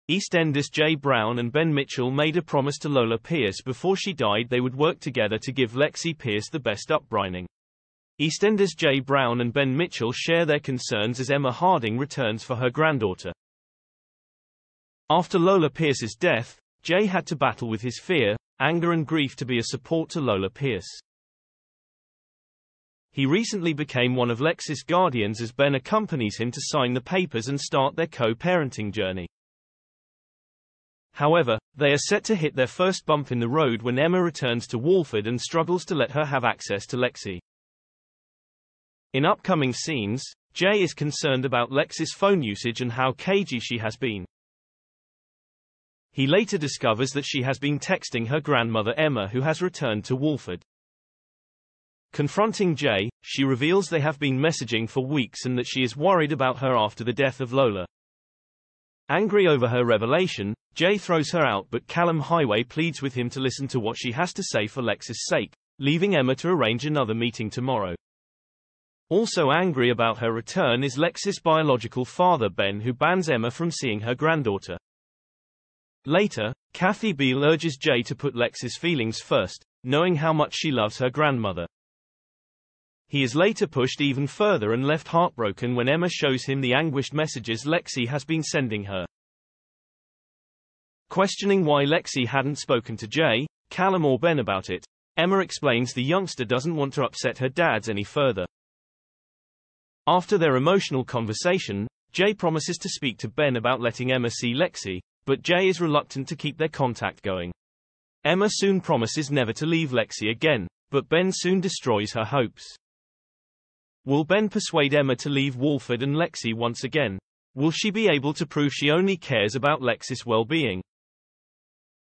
EastEnders' Jay Brown and Ben Mitchell Ieft to make huge decision over Lexi Pearce's future
EastEnders's Jay Brown and Ben Mitchell made a promise to Lola Pearce before she died they would work together to give Lexi Pearce the best upbrining.